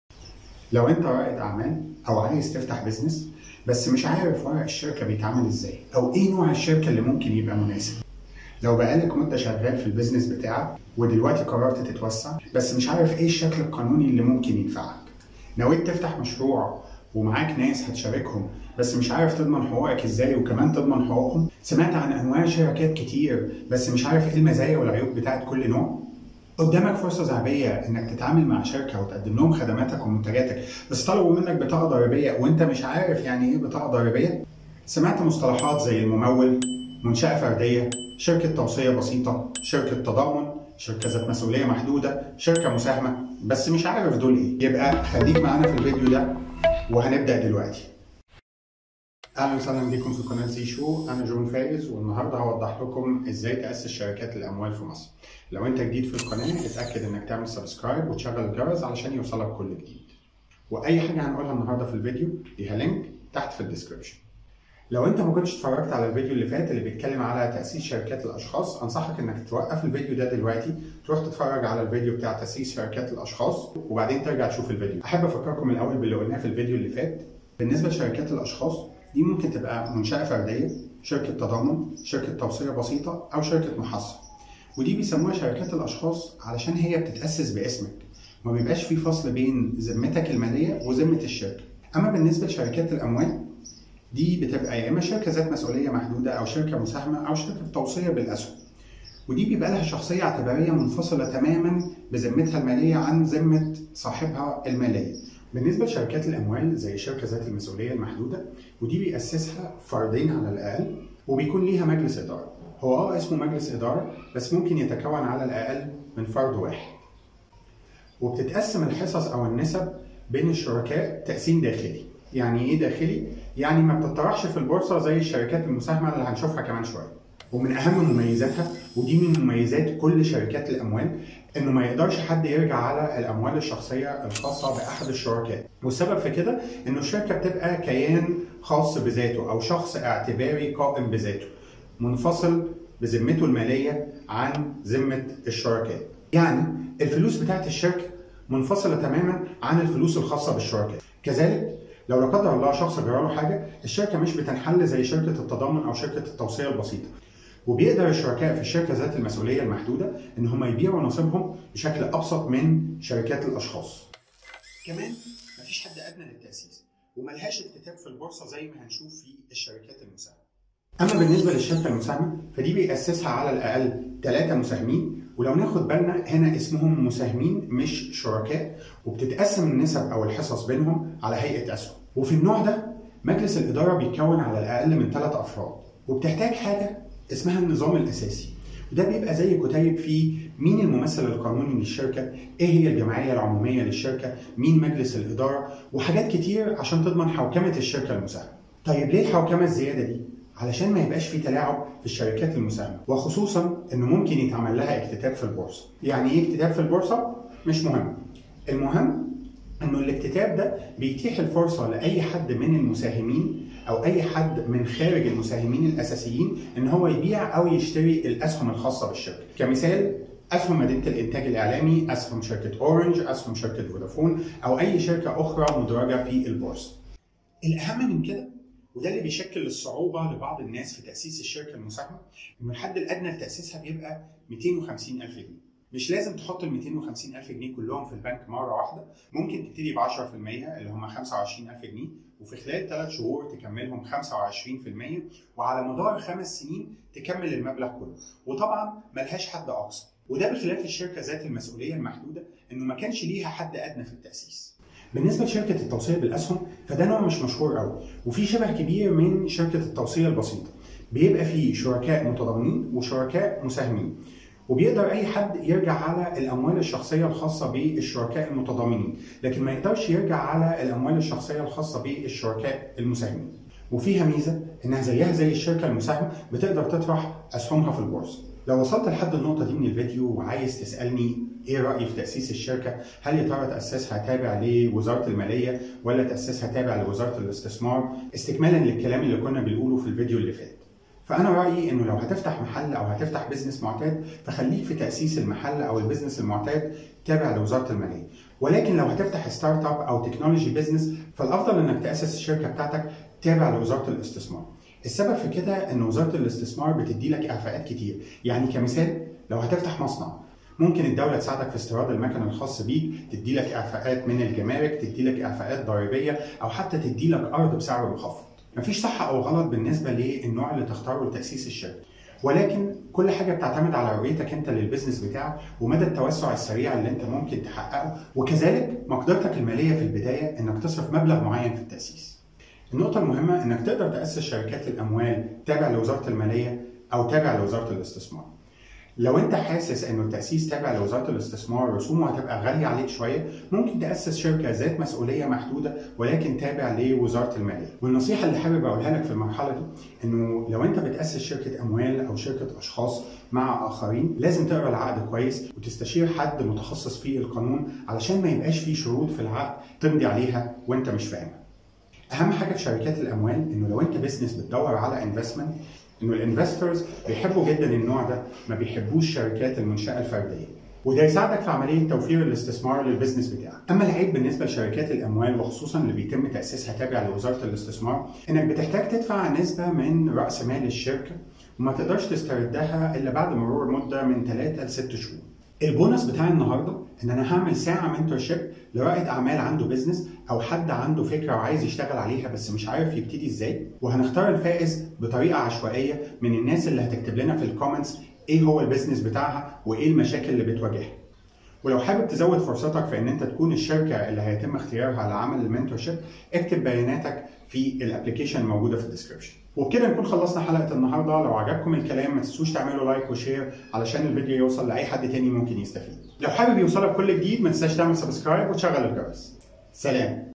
رواد الاعمال في مصر: ( اشكال الشركات 2020 ) شركة الاموال
لكل رواد الاعمال في مصر فإن اشكال الشركات يمكن أن تؤسس كشركة الاشخاص او شركة الاموال. بالنسبة لشركة الاموال وهي موضوع هذا الفيديو فيمكن أن تكون شركة ذات مسئولية محدودة او شركة مساهمة مصرية او شركة توصية بالاسهم. هذا الفيديو هو فيديو مهم به نصائح لكل رواد الاعمال في مصر ومن يرغب في تأسيس الشركات خصوصا تأسيس ستارت اب او تأسيس شركة ناشئة فهو يوضح  شركة الاموال والاشخاص والفرق بينهم

 في هذا الفيديو تعرف على اشكال الشركات وتأسيس الشركات في مصر خصوصا شركة الاموال فقد تكون الشركة ذات المسئولية المحدودة والتي تحتاج الي 2 شركاء علي الاقل لتأسيسها وتقسم النسب والحصص في هذه الشركة تقسيم داخلي ولا تمتد المسئولية القانونيه للاموال الشخصية الخاصة بالمؤسسين لأن الشركة تتمتع بكيان اعتباري منفصل من ضمن العديد من المزايا الاخري التي يمكنك التعرف عليها في الفيديو

اما بالنسبة للشركه المساهمة وهي شكل اخر من اشكال الشركات (شركة الاموال) وهي تحتاج علي الاقل الي 3 مساهمين وتسمي النسب اسهم. وفي هذا النوع يجب وجود مجلس الادارة من 3 اعضاء علي الاقل وكذلك وجود النظام الاساسي للشركة والذي يضمن الحوكمة الجيدة للشركة. أما بالنسبة للمبلغ المالي المطلوب لتأسيس الشركة المساهمة هو 250 الف جنيه يقوم رواد الاعمال في مصر بسدادها بطريقة معينه اثناء عملية تأسيس الشركة تبعا قانون الاستثمار الجديد. كما انه يمكن ان تكتتب الشركة في البورصه لبيع لأفراد من خارج المساهمين الرئيسين.

الشكل الثالث من اشكال شركة الاموال هو شركة التوصية بالاسهم والتي تشبه شركة التوصية البيسطة في بعض الخواص و الشركة المساهمة في بعض الخواص الاخري. فهي شركة اموال يمكن أن تطرح اسهم في البورصه. كما أن بها شركاء متضامنون وشركاء مساهمون. الشركاء المتضامنون مثل نظيره في شركة التضامن (شركة اشخاص ) والتي قمنا بتوضيحها في الفيديو السابق يمكن الرجوع علي امواله الخاصه لسداد ديون شركة التوصية بالاسهم اما الشريك المساهم فإن امواله الخاصه ليس لها علاقة بالشركة.

في نهاية الفيديو يوجد العديد من النصائح الخاصه بأشكال الشركات سواء شركة الاموال او شركة الاشخاص. وهي نصائح مهمة لتأسيس شركة ناشئة مرنه (شركة ستارت اب او شركة صغيره). كذلك هذه النصائح مفيدة لكل رواد الاعمال في مصر. وتوضح الفرق بين انواع الشركات في مصر (اشكال الشركات 2020) وتأسيسها ، الفرق بين القانون التجاري المصري وقانون الاستثمار الجديد

يمكنك تأسيس الشركة تابعة لقانون الاستثمار الجديد للاستفادة من الاعفاءات التي تتيحها الدولة لكل رواد الاعمال في مصر من خلال عملية تأسيس شركة الاموال تابع لوزارة الاستثمار.

تسهيلا علي كل رواد الاعمال في مصر عشان يتعرفو علي اشكال الشركات حطينا توقيتات الفيديو كالتالي
Video TimeStamp (توقيتات الفيديو)

0:00 Reasons why you should watch this video (اسباب لمشاهدة فيديو شركة الاموال)
0:50 Introduction المقدمة
1:04 Message to watch Part one   التوجيه لمشاهدة الجزء الاول
1:28 Introduction المقدمة
1:16 Personal Companies  شركة الاشخاص
1:30 Monetary Companies شركة الاموال
1:45 Limited Liability Company شركة ذات مسئولية محدودة
2:53 Shareholding Company الشركة المساهمة
4:14 Joint venture company by shares شركة التوصية بالاسهم
4:37 Advice for Egyptian Entrepreneurs نصائح رواد الاعمال في مصر
6:25 Video Bonus and question  بونص الفيديو وسؤال الحلقة